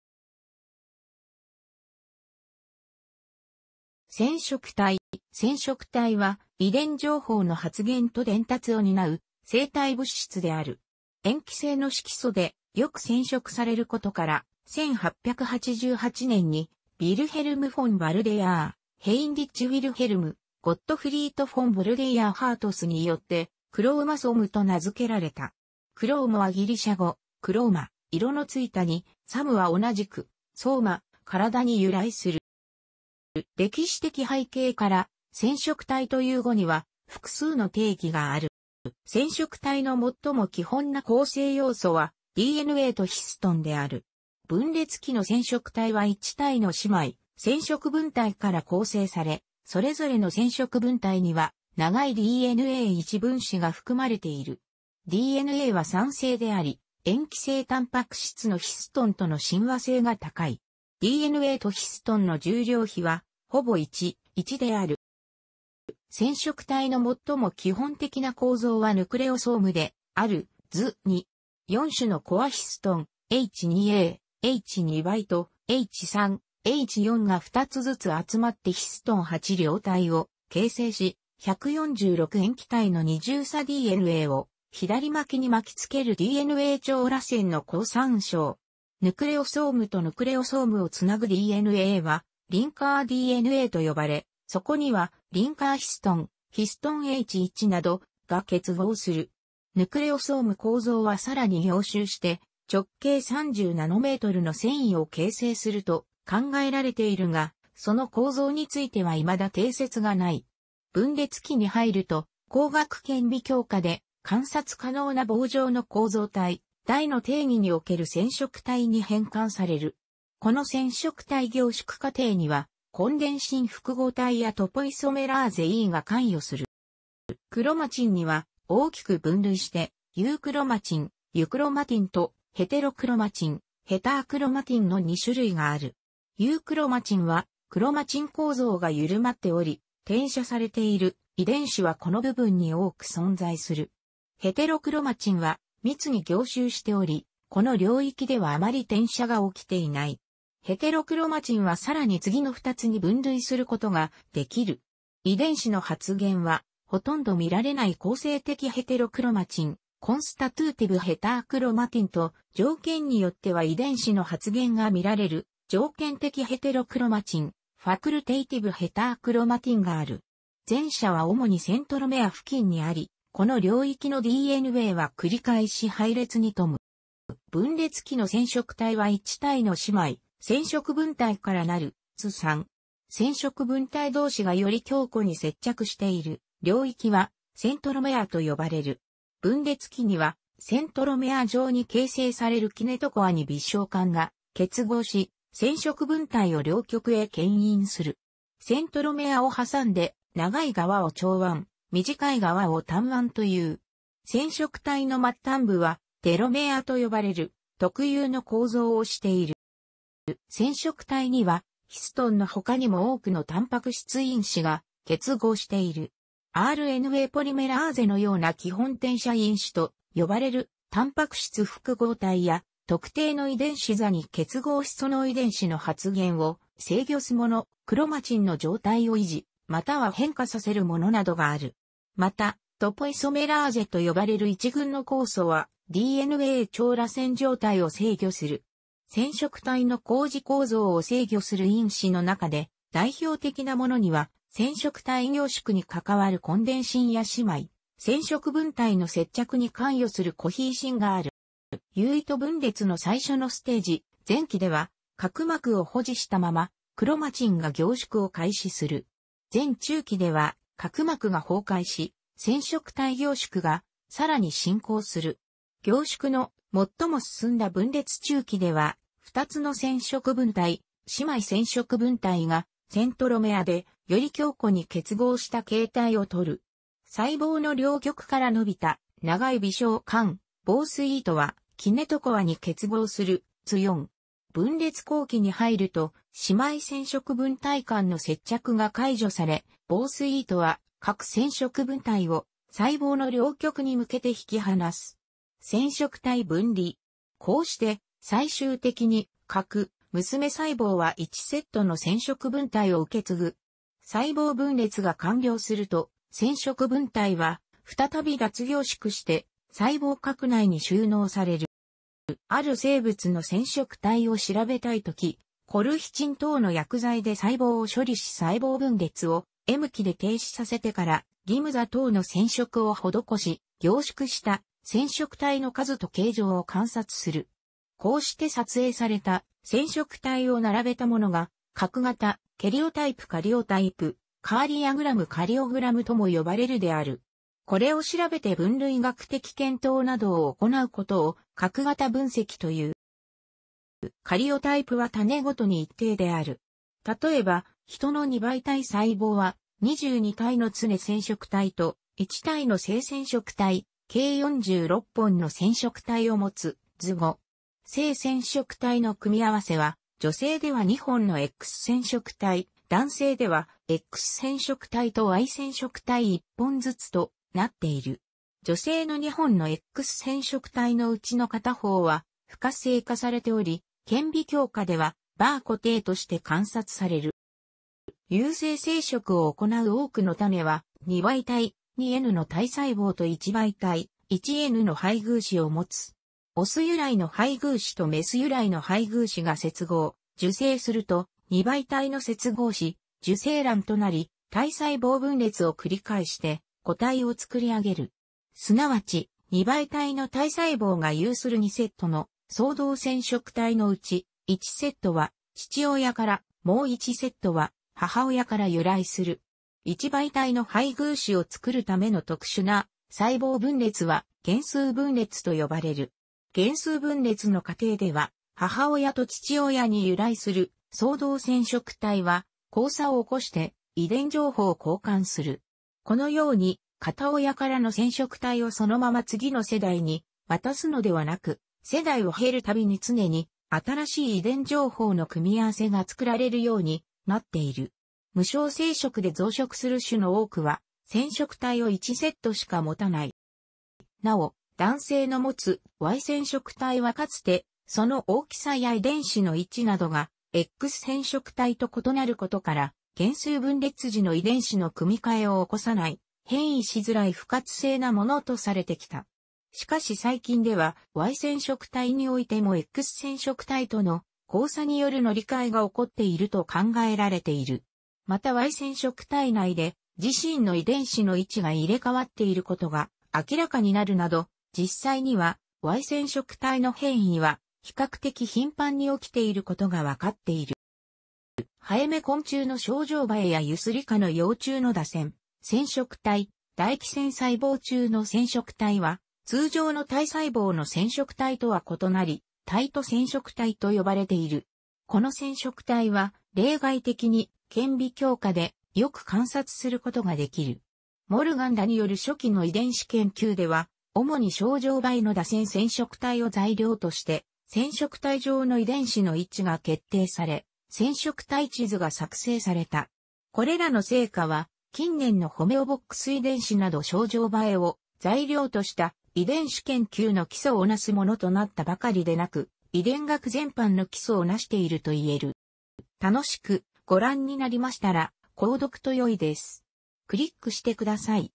染色体
染色体（せんしょくたい）は遺伝情報の発現と伝達を担う生体物質である。塩基性の色素でよく染色されることから、1888年にヴィルヘルム・フォン・ヴァルデヤー(Heinrich Wilhelm Gottfried von Waldeyer-Hartz)によって Chromosome と名付けられた。"Chromo-" はギリシャ語 ("chroma") 「色のついた」に、"-some" は同じく ("soma") 「体」に由来する。

歴史的背景から、染色体という語には複数の定義がある。

染色体の最も基本な構成要素は、DNAとヒストンである。分裂期の染色体は一対の姉妹染色分体から構成され、それぞれの染色分体には長いDNA一分子が含まれている。DNAは酸性であり、塩基性タンパク質のヒストンとの親和性が高い。DNAとヒストンの重量比は、ほぼ1:1である。

染色体の最も基本的な構造はヌクレオソームである（図2）。4種のコアヒストン（H2A, H2B, H3, H4）が2つずつ集まってヒストン8量体を形成し、146塩基対の2重鎖DNAを左巻きに巻きつける（DNA超らせんの項参照）。ヌクレオソームとヌクレオソームをつなぐDNAはリンカーDNAと呼ばれ、そこにはリンカーヒストン（ヒストンH1など）が結合する。ヌクレオソーム構造はさらに凝集して直径30 nmの繊維を形成すると考えられているが、その構造についてはいまだ定説がない。分裂期にはいると、光学顕微鏡下で観察可能な棒状の構造体（第一の定義における染色体）に変換される。この染色体凝縮過程には、コンデンシン複合体やトポイソメラーゼ II が関与する。

クロマチンには、大きく分類してユークロマチン (euchromatin) とヘテロクロマチン (heterochromatin) の二種類がある。ユークロマチンはクロマチン構造がゆるまっており、転写されている遺伝子はこの部分に多く存在する。ヘテロクロマチンは密に凝集しており、この領域ではあまり転写が起きていない。ヘテロクロマチンは更に次の二つに分類することができる。遺伝子の発現はほとんど見られない構成的ヘテロクロマチン (constitutive heterochromatin) と、条件によっては遺伝子の発現が見られる条件的ヘテロクロマチン (facultative heterochromatin) がある。前者は主にセントロメア付近にあり、この領域の DNA は繰り返し配列に富む。

分裂期の染色体は一対の姉妹染色分体からなる（図3）。染色分体どうしがより強固に接着している領域はセントロメアと呼ばれる。分裂期にはセントロメア上に形成されるキネトコアに微小管が結合し、染色分体を両極へ牽引する。セントロメアをはさんで長い側を長腕、短い側を短腕という。染色体の末端部はテロメアと呼ばれる特有の構造をしている。

染色体にはヒストンの他にも多くのタンパク質因子が結合している。RNAポリメラーゼのような基本転写因子と呼ばれるタンパク質複合体や、特定の遺伝子座に結合しその遺伝子の発現を制御するもの、クロマチンの状態を維持または変化させるものなどがある。また、トポイソメラーゼと呼ばれる一群の酵素は、DNA超らせん状態を制御する。染色体の高次構造を制御する因子の中で代表的なものには、染色体凝縮に関わるコンデンシンや姉妹染色分体の接着に関与するコヒーシンがある。

有糸分裂の最初のステージ（前期）では、核膜を保持したまま、クロマチンが凝縮を開始する。前中期では、核膜が崩壊し、染色体凝縮がさらに進行する。凝縮の最も進んだ分裂中期では、二つの染色分体（姉妹染色分体）がセントロメアでより強固に結合した形態をとる。細胞の両極から伸びた長い微小管（紡錘糸）はキネトコアに結合する（図4）。分裂後期にはいると、姉妹染色分体間の接着が解除され、紡錘糸は各染色分体を細胞の両極に向けて引き離す。（染色体分離）。こうして、最終的に各娘細胞は1セットの染色分体を受け継ぐ。細胞分裂が完了すると、染色分体は再び脱凝縮して細胞核内に収納される。

ある生物の染色体を調べたいとき、コルヒチン等の薬剤で細胞を処理し細胞分裂をM期で停止させてからギムザ等の染色を施し、凝縮した染色体の数と形状を観察する。こうして撮影された染色体を並べたものが、核型（karyotype カリオタイプ、karyogram カリオグラム とも呼ばれる）である。これを調べて分類学的検討などを行うことを核型分析という...